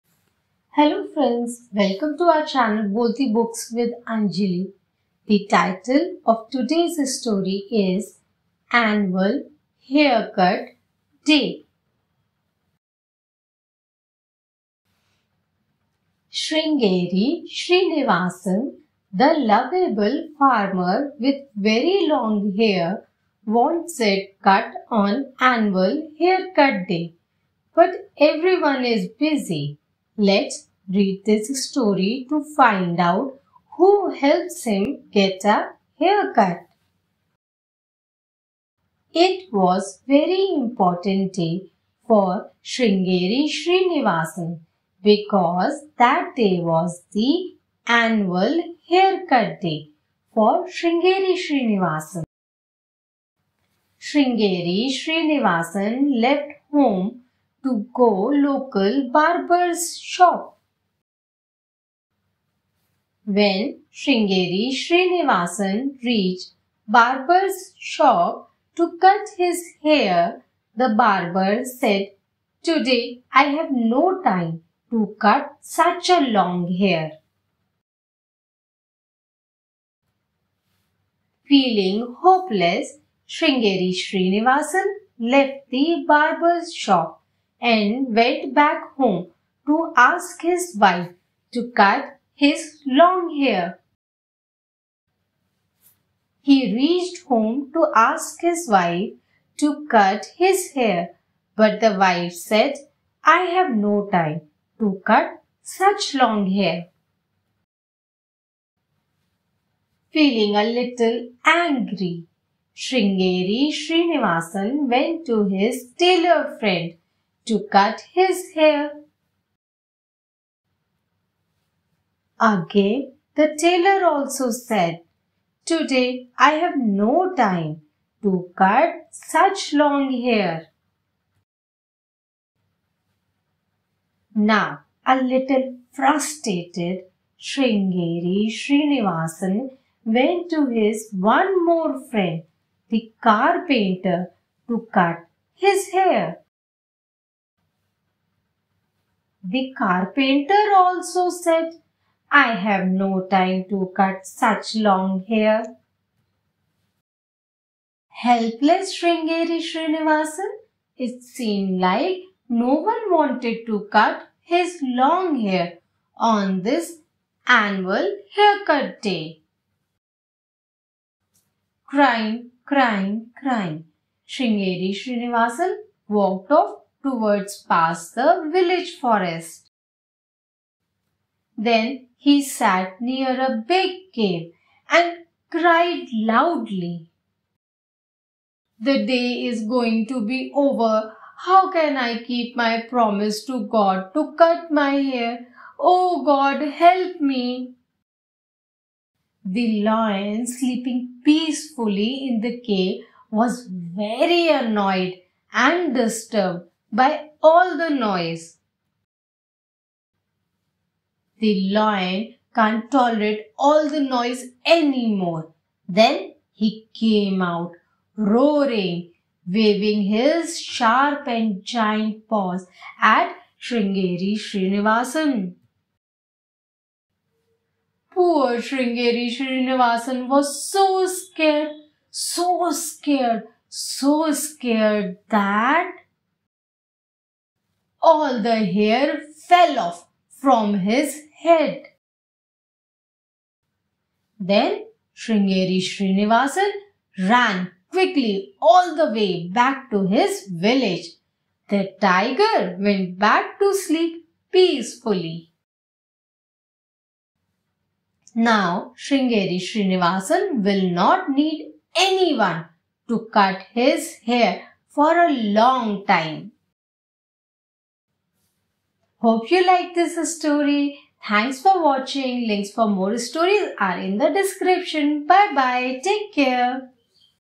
Annual haircut day. A funny story in English. Read aloud-read along.
This is a story about a farmer who has very long hair. He cuts it once every year, and today is that auspicious day. But he can't find anyone to cut it. Help comes from an unexpected source. Read this story and have lots of fun.